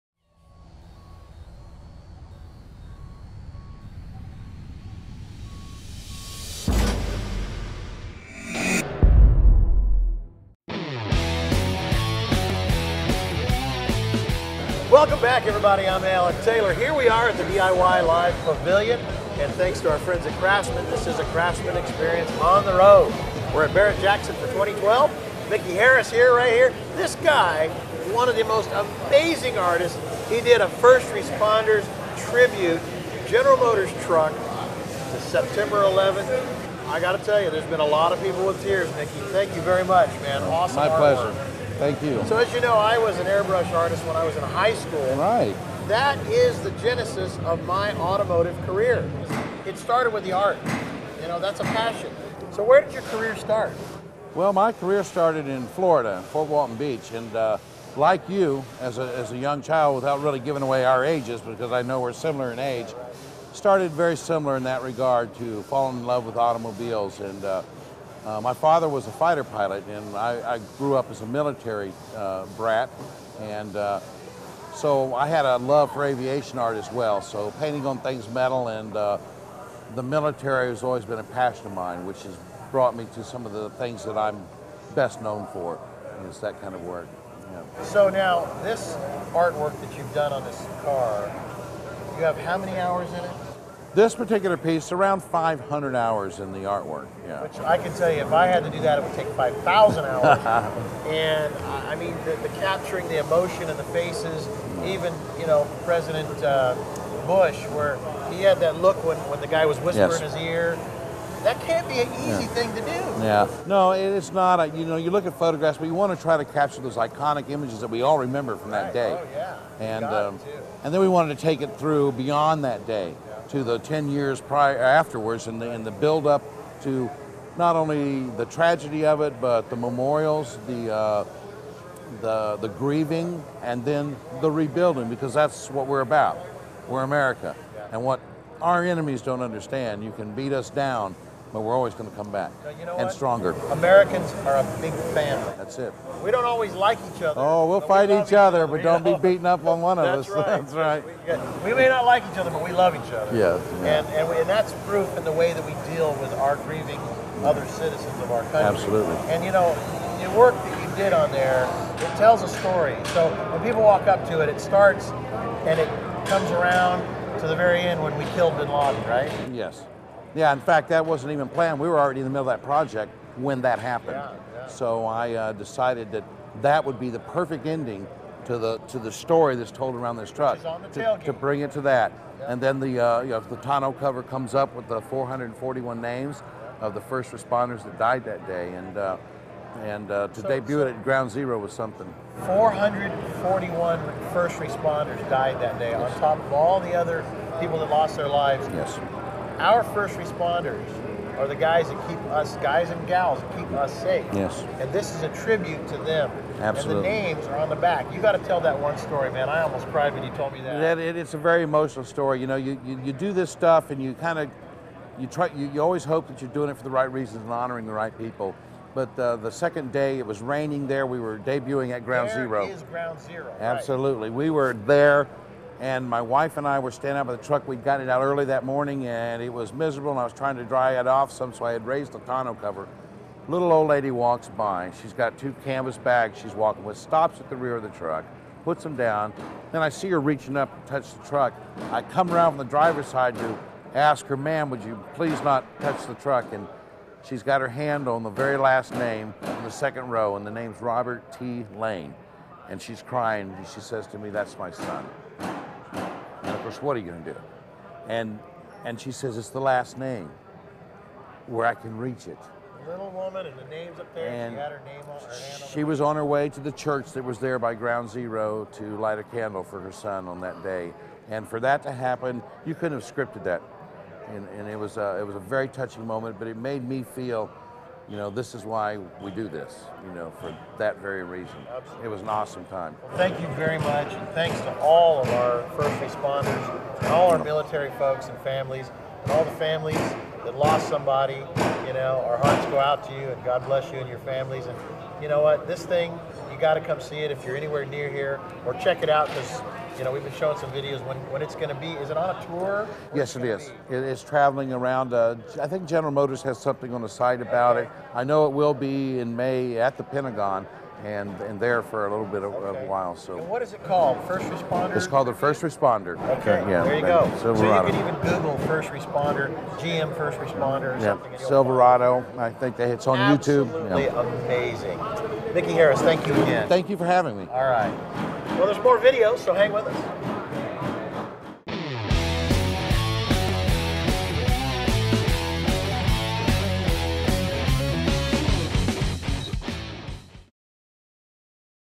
Barrett-Jackson DIY Pavilion: 9/11 First Responders Truck
Host Alan Taylor talks with airbrush artist Mickey Harries about his tribute to the 9/11 first responders - a GM truck covered in an airbrushed mural.